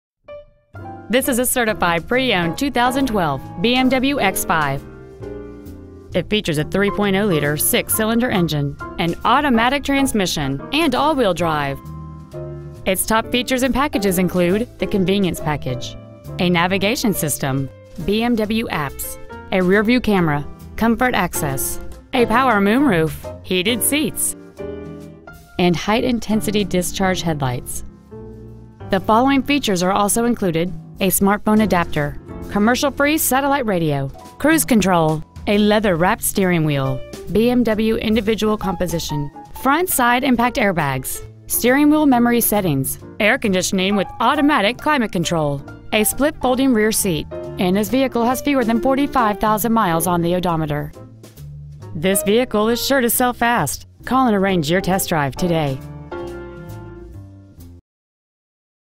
Certified 2012 BMW X5 xDrive35i Beverly Hills CA 90036
Year : 2012
 Make : BMW
 Model : X5 xDrive35i
 Engine : 3.0L I-6 cyl
 Trans . : Automatic
 Exterior : Space Gray
 Miles : 44,285
 Interior : Black

 Preowned 2012 BMW X5 xDrive35i Beverly Hills CA